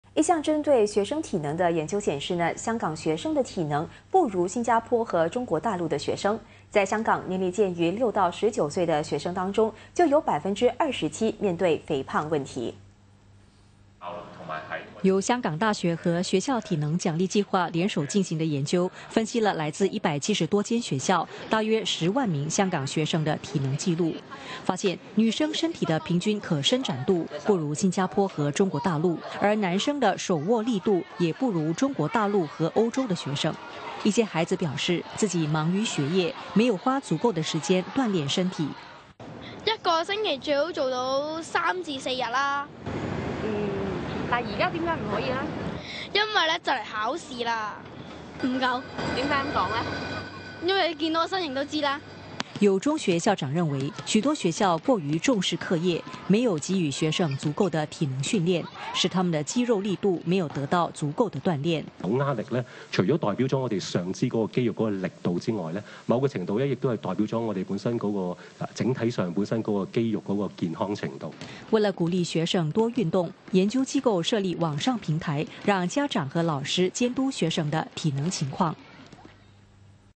研究显示香港学生运动不足 体能比新加坡和
一项针对学生体能的研究显示，香港学生的体能，不如新加坡和中国大陆的学生。在香港，年龄介于六到19岁的学生当中，就有27%面对肥胖问题。